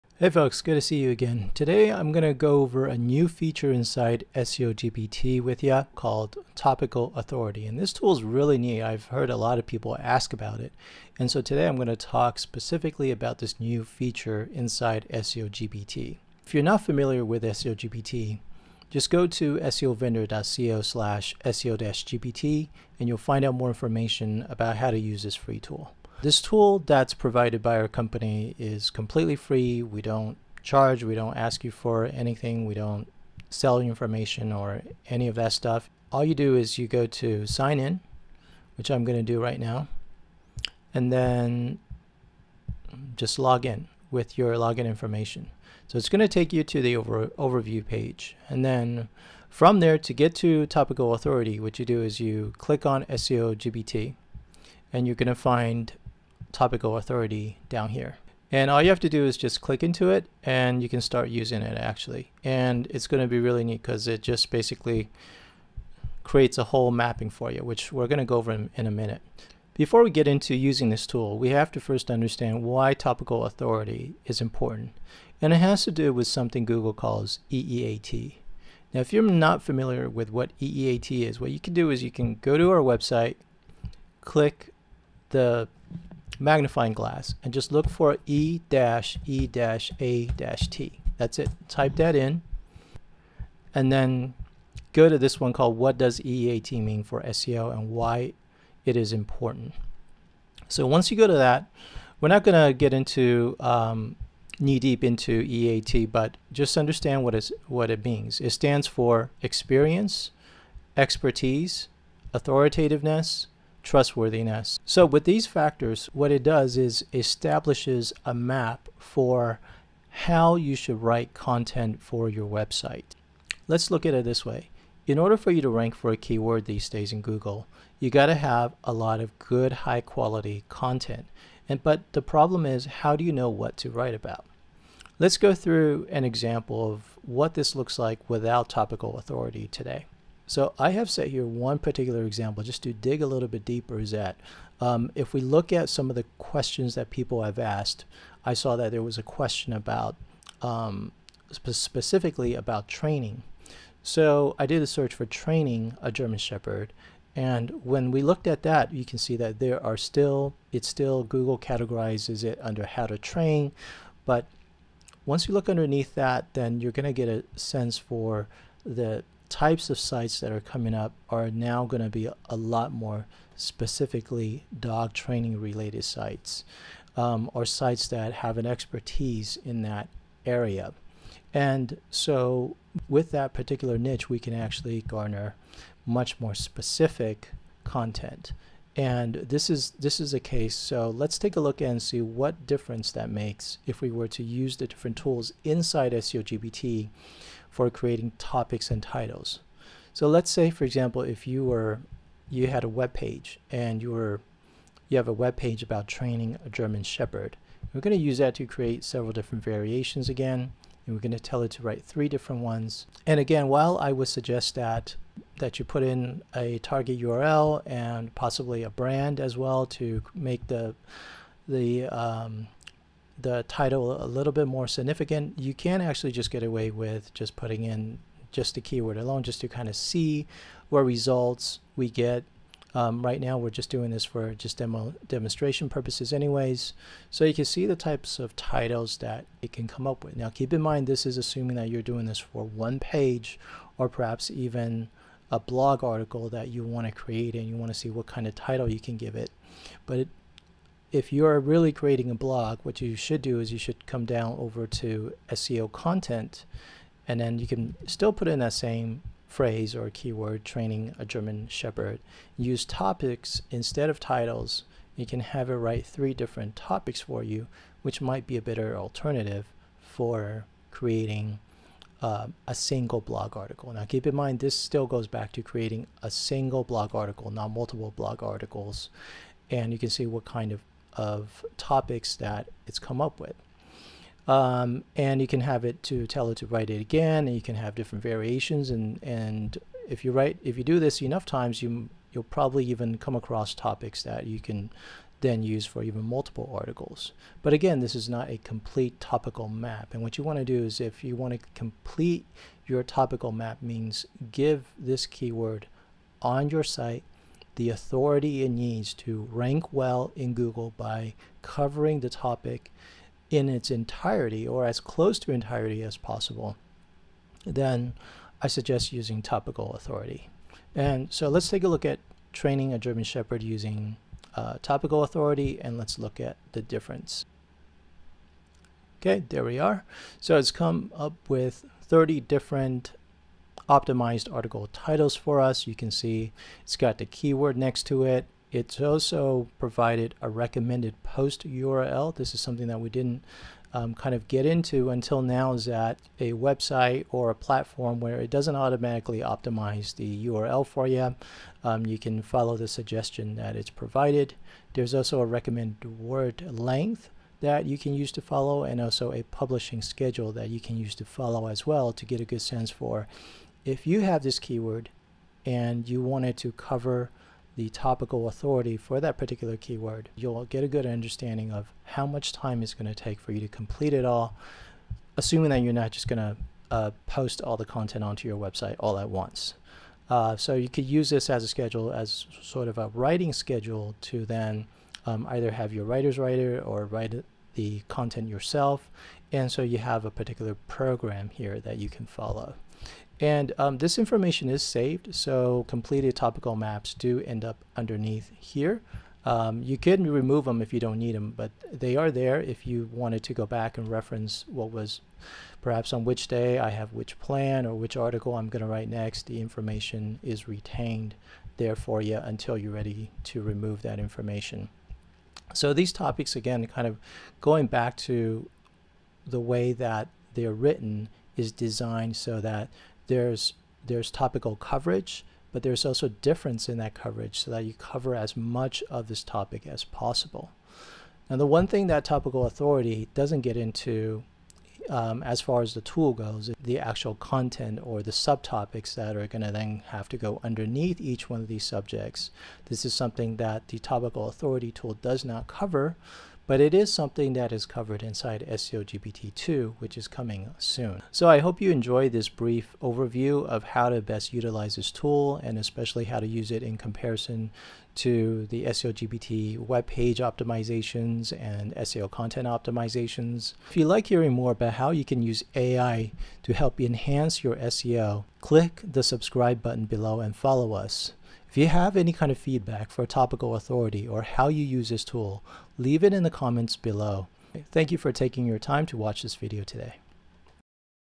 AI SEO: Shocking Truth About (Topical Authority Super Tool)
You need to watch this on Topical Authority if you want to know how far AI has come.   This method CANNOT be duplicated with ChatGPT either!

It's an important concept for any webmaster or SEO expert looking to gain a competitive edge and higher ranks in search engine results. This video will explain how the Topical Authority tool can be used by website owners to improve their rankings with major players like Google, Bing, and Yahoo!

Along the way you'll learn key strategies that will help you become more informed on how content marketing works with respect to gaining relevant E-E-A-T ranking signals within these search engines. We'll also cover tips on creating engaging content which is methodically tailored specifically towards your target audience - crucial information if you want greater visibility over time.

Finally, there are some common mistakes made when it comes to implementing successful tactics designed around building up Topical Authority- so make sure not to miss out on learning those as well.. So join us now...as we take a deep dive into understanding what Topical Authority really means for online marketers looking boost visibility via improved Rankings!

(This description was written by the SEO GPT)